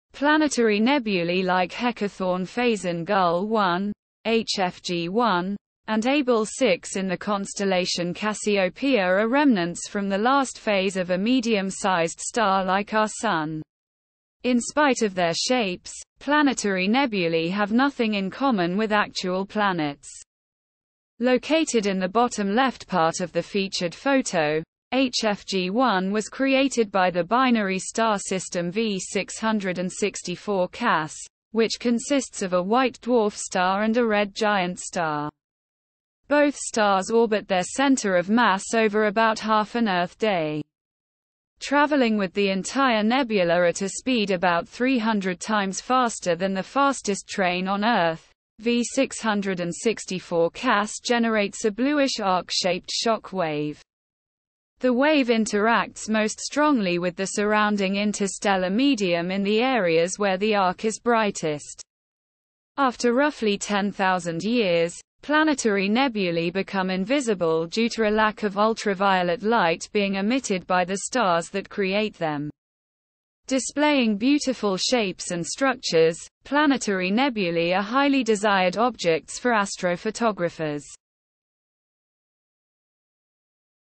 HFG1 & Abell 6: Planetary Nebulae
Planetary nebulae like Heckathorn-Fesen-Gull 1 (HFG1) and Abell 6 in the constellation Cassiopeia are remnants from the last phase of a medium sized star like our Sun.  In spite of their shapes, planetary nebulae have nothing in common with actual planets.  Located in the bottom left part of the featured photo, HFG1 was created by the binary star system V664 Cas, which consists of a white dwarf star and a red giant star. Both stars orbit their center of mass over about half an Earth day.  Traveling with the entire nebula at a speed about 300 times faster than the fastest train on Earth, V664 Cas generates a bluish arc shaped shock wave. The wave interacts most strongly with the surrounding interstellar medium in the areas where the arc is brightest. After roughly 10,000 years, planetary nebulae become invisible due to a lack of ultraviolet light being emitted by the stars that create them.  Displaying beautiful shapes and structures, planetary nebulae are highly desired objects for astrophotographers.

Image Credit & Copyright:
Julien Cadena &
Mickael Coulon;
Text: Natalia Lewandowska
(SUNY Oswego)